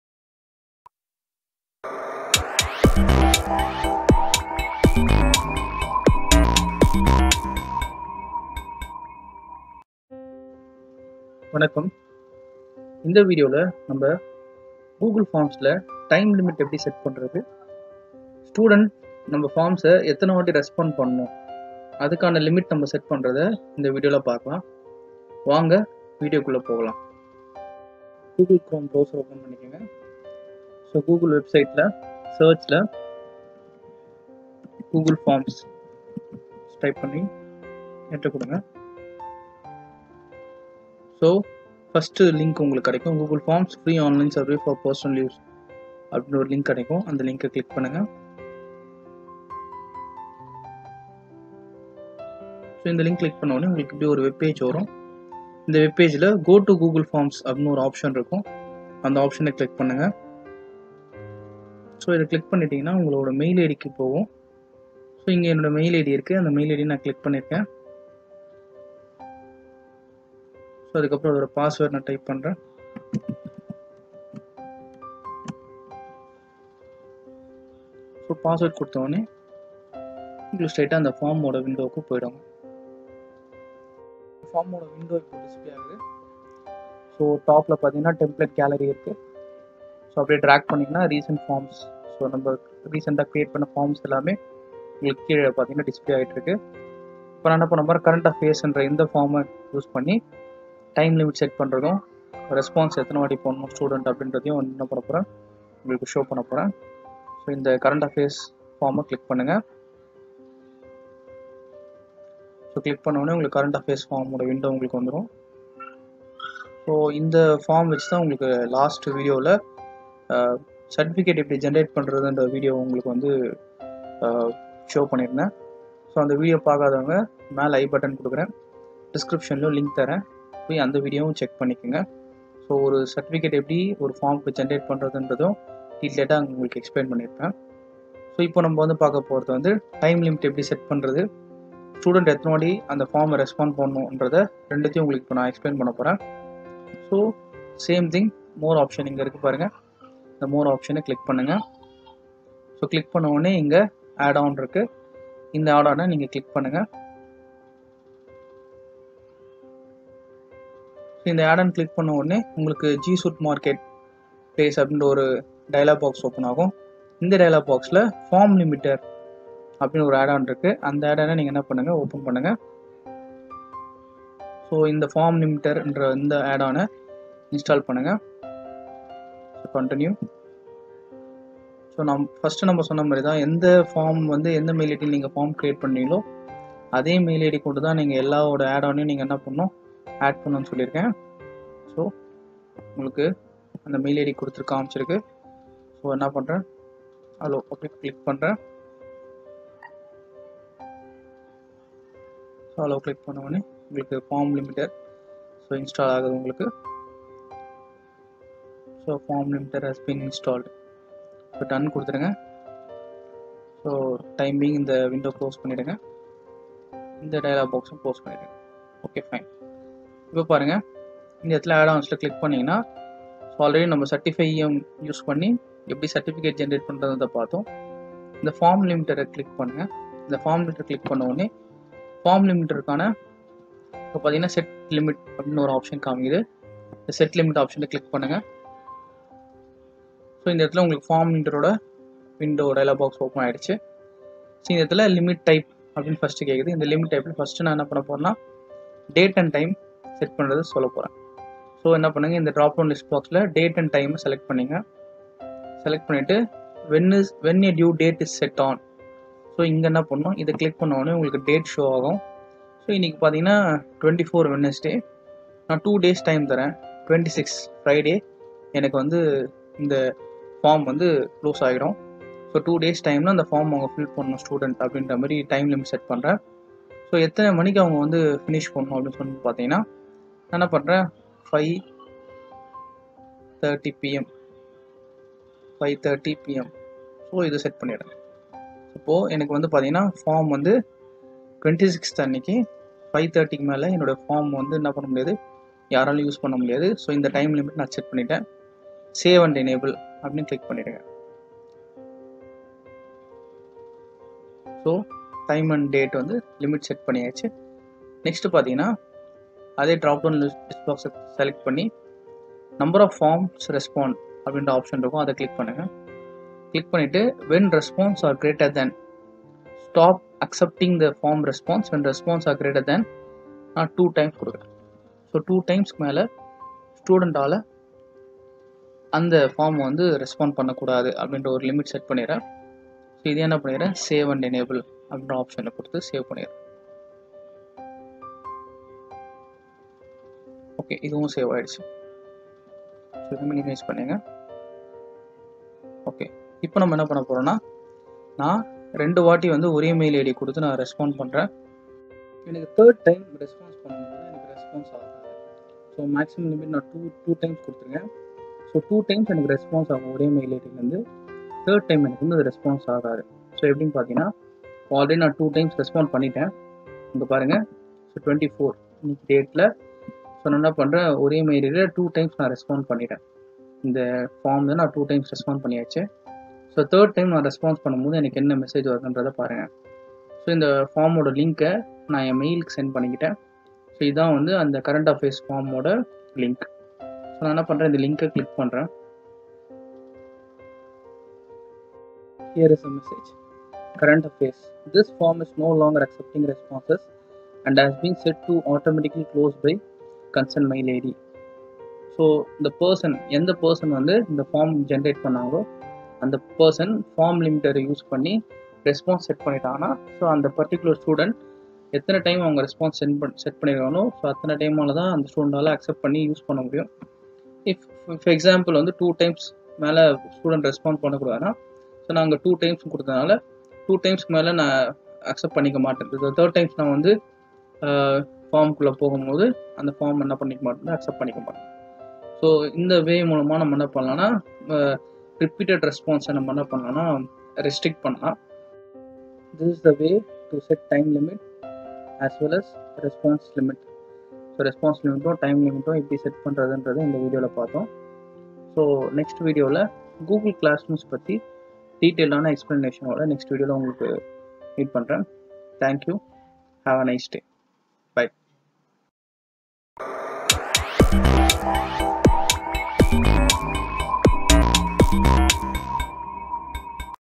How to Set Time Limit and Response Time in Google Forms Just 3 Mins in Tamil
This Video explain about how to set time limit and response limit in Google Forms in Tamil. Other google form related video also posting below. Kindly check it.

Sample Form to Generate E- Certificate to your Mail. Just Look at your Certificate